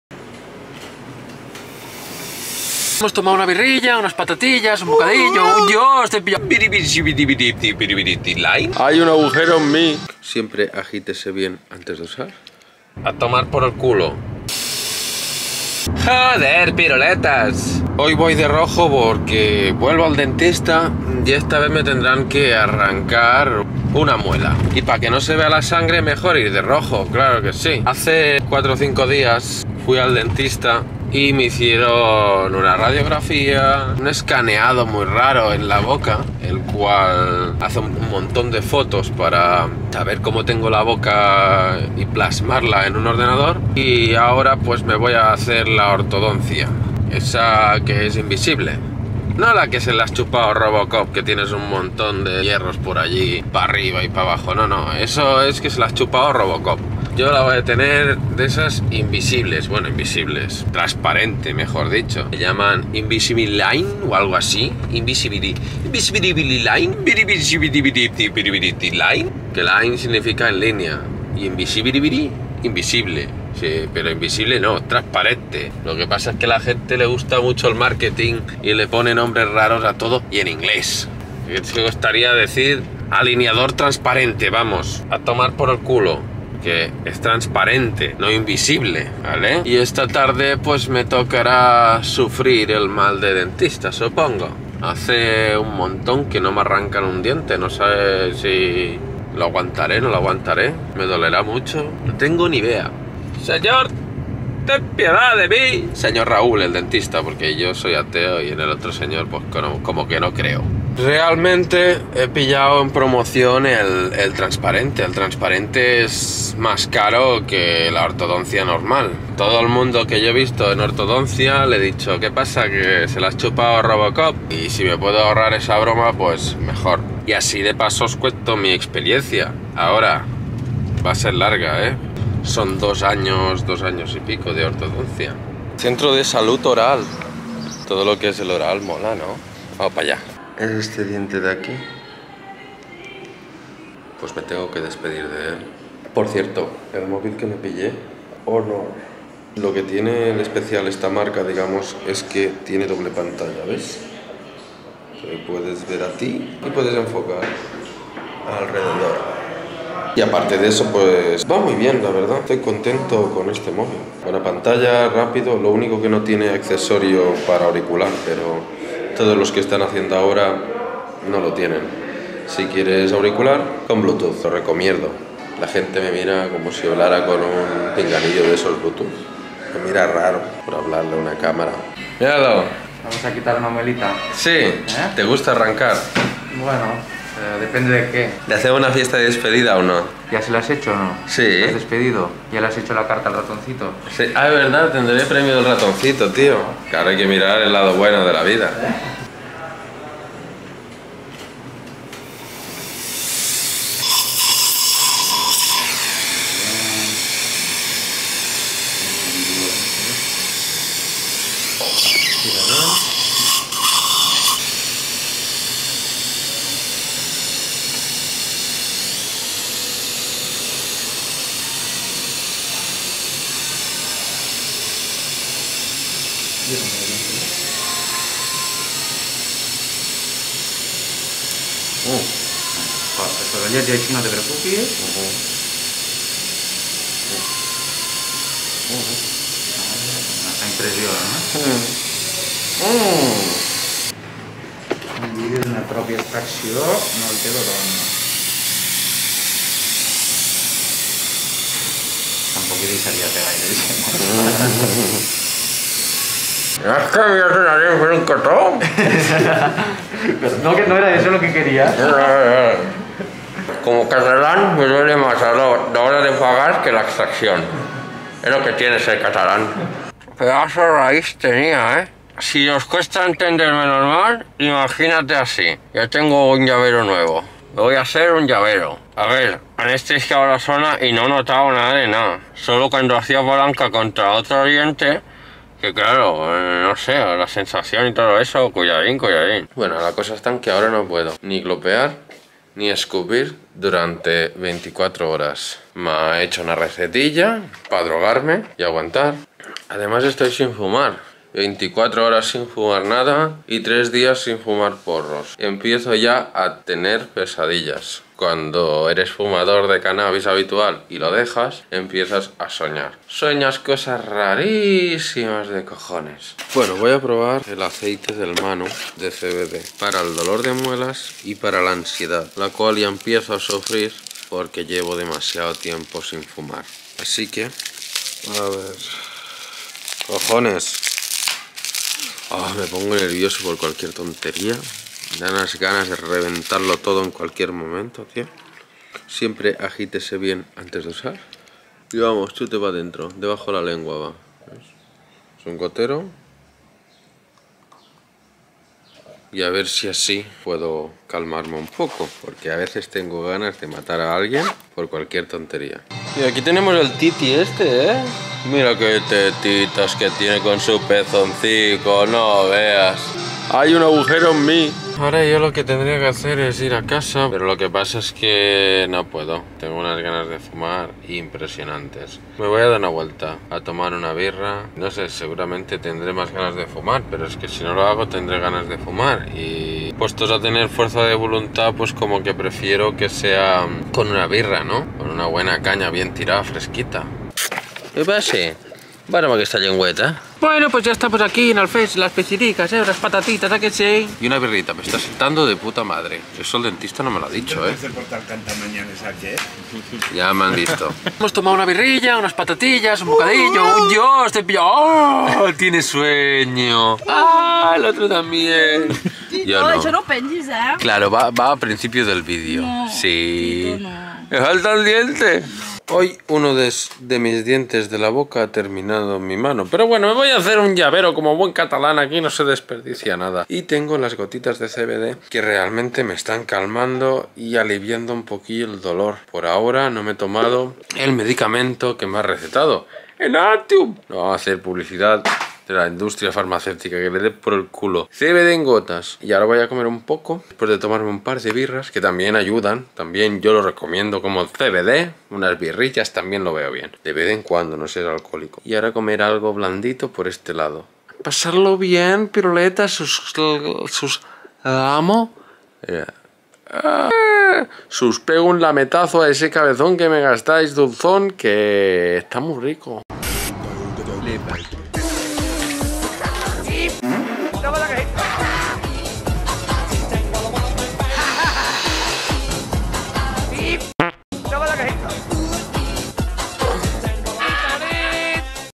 Ratoncito Pérez Dame Mi Pasta💸 , Un Poquito de CBD y Un Nuevo Llavero 🦷-DIA 39- WOODY TIENE UN PLAN
MISTER WOODY